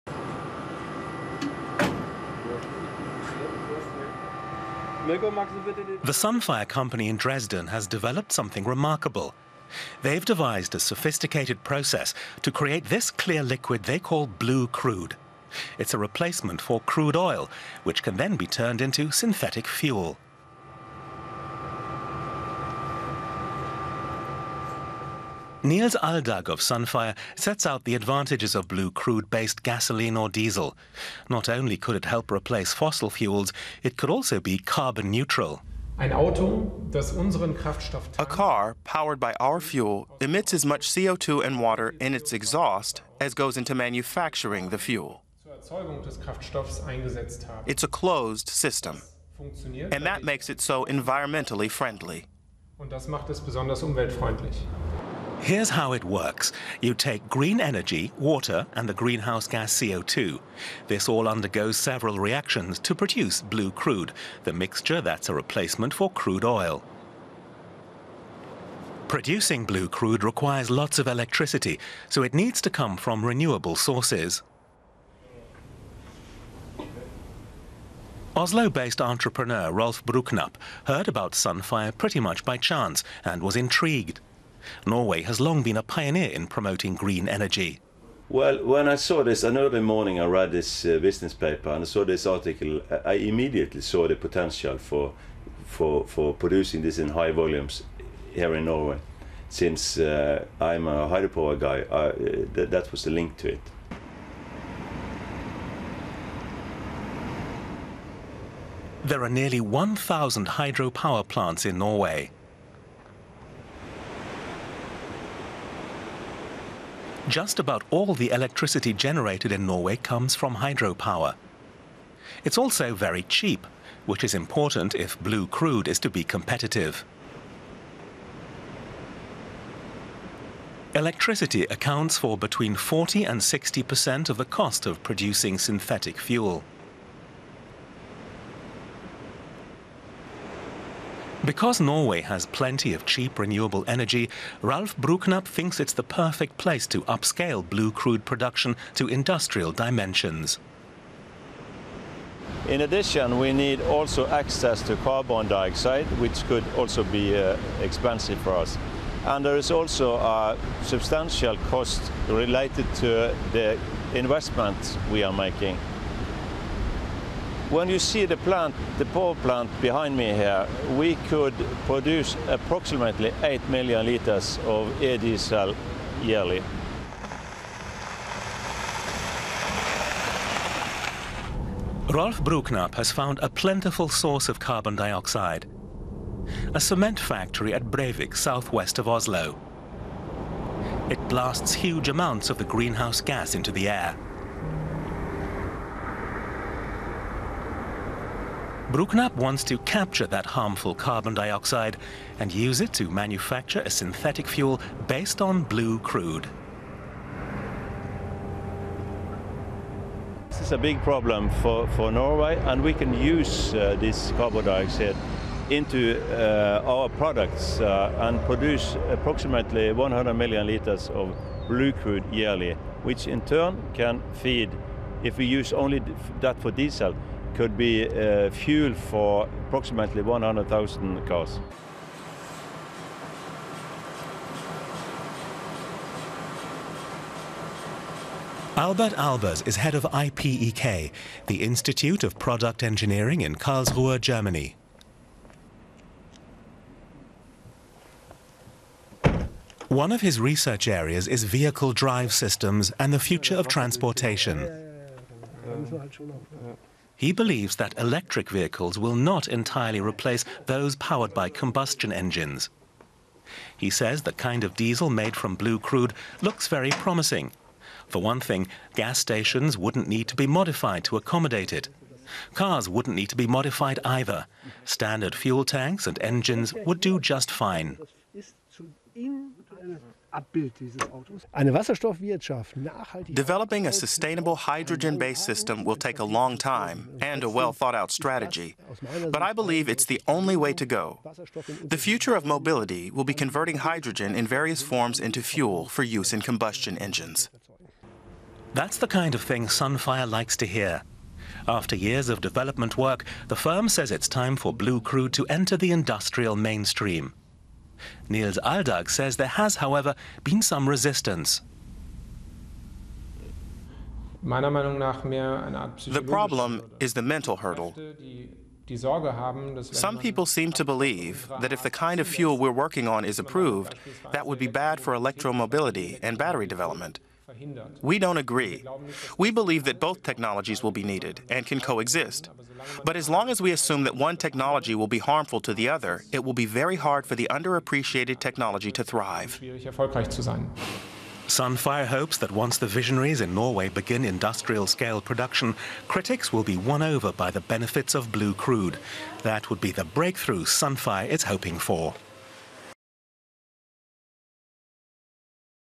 Sunfire - The search for new fuels | DW English
The Sunfire company in Dresden has developed an environmentally friendly way to make diesel out of just carbon dioxide, renewable electricity and water. It's a new twist in both the e-mobility and combustion-engine story.